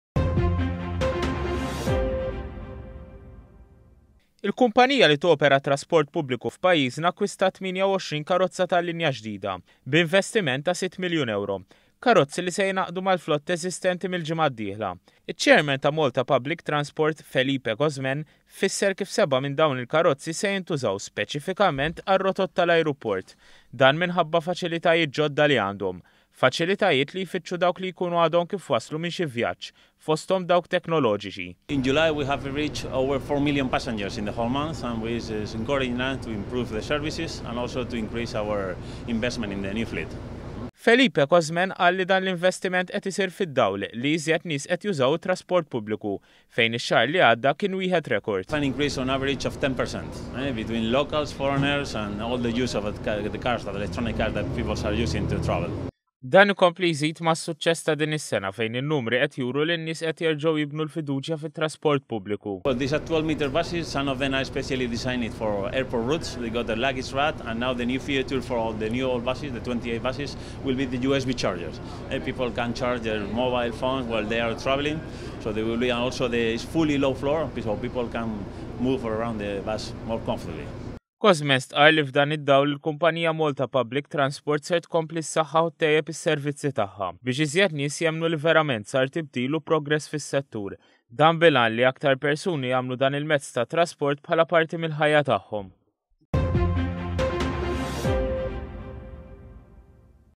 INVESTIMENT TA’ €6,000,000 F’KAROZZI ĠODDA.
ONE.COM.MT • Ġew inawgurati tmienja u għoxrin karozza tal-linja ġdida dan bil-għan li jiżdiedu s-servizzi u aktar nies jużaw it-trasport pubbliku. Filfatt turija ta’ kemm is-servizz tkib huwa x-xahar l-ieħor hekk kif kien wieħed rekord fejn erba miljun persuna għamlu użu mit-trasport pubbliku.